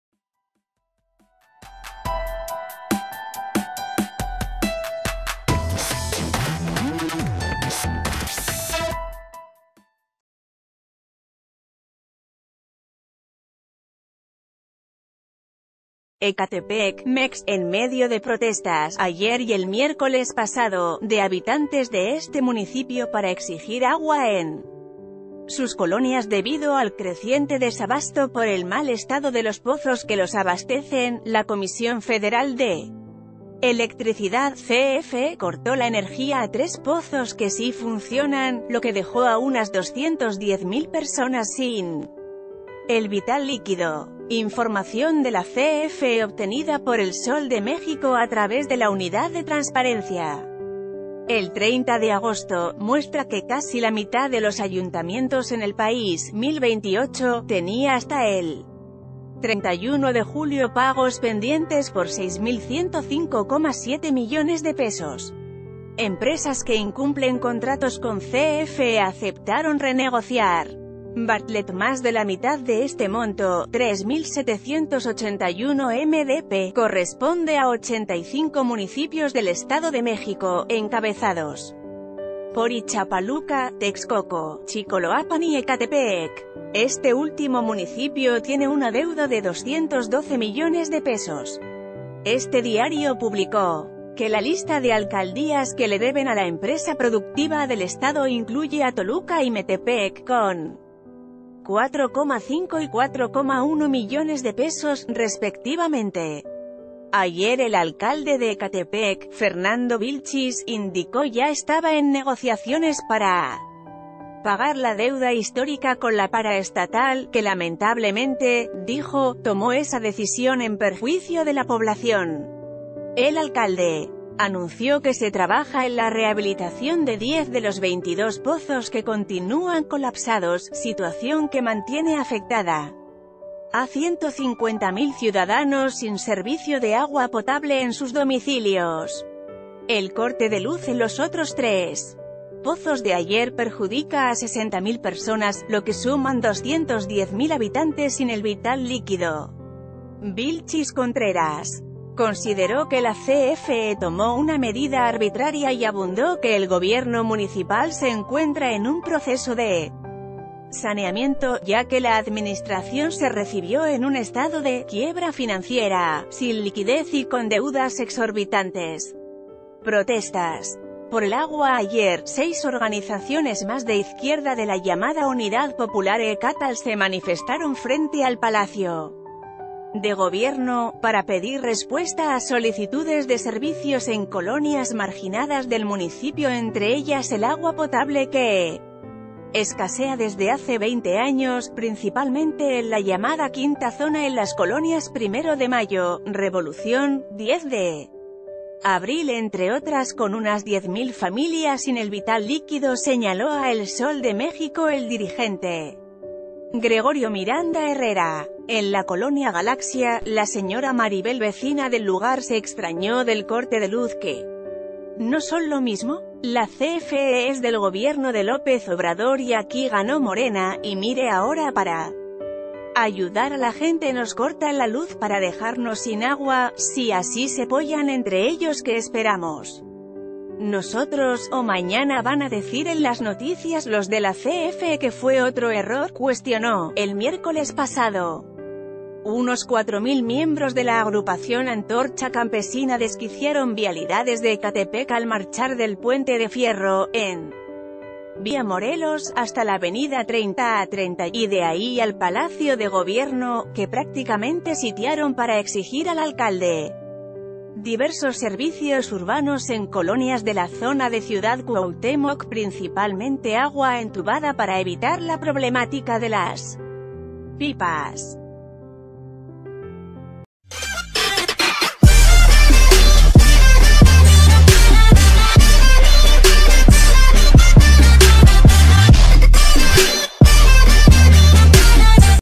CFE corta la luz a pozos de Ecatepec, afecta a 210 mil personas
ECATEPEC, Méx.- En medio de protestas (ayer y el miércoles pasado) de habitantes de este municipio para exigir agua en sus colonias debido al creciente desabasto por el mal estado de los pozos que los abastecen, la Comisión Federal de Electricidad (CFE) cortó la energía a tres pozos que sí funcionan...